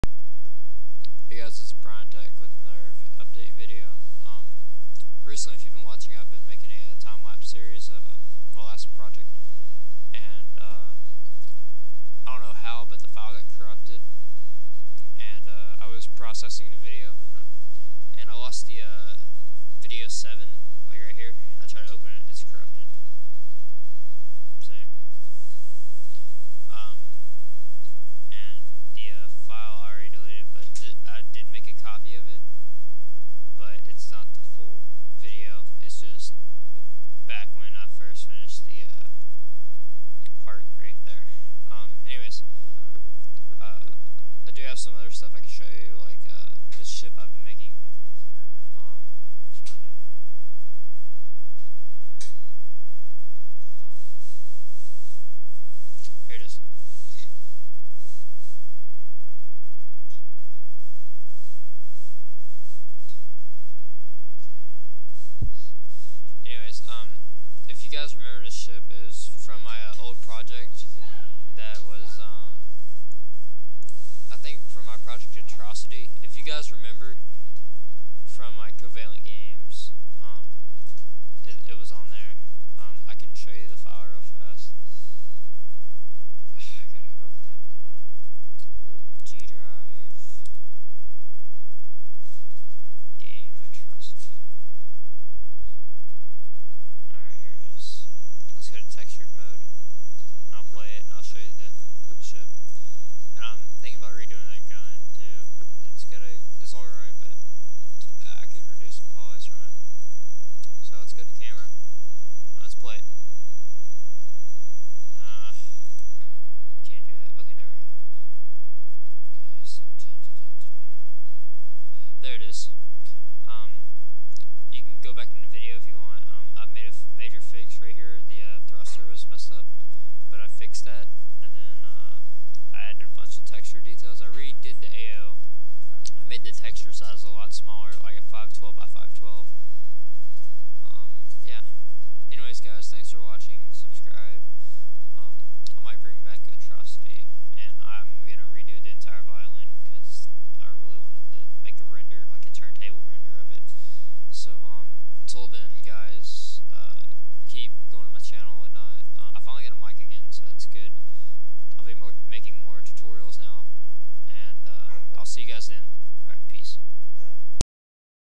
xBryanTechx Update
I hate to say but my violin file got corrupt and i was starting to texture it i modeled the entire violin in part 7. anyways I was working on a ship from the atrocity project which i might bring back (: anyways you know what to do subscribe if you wanna stay updated.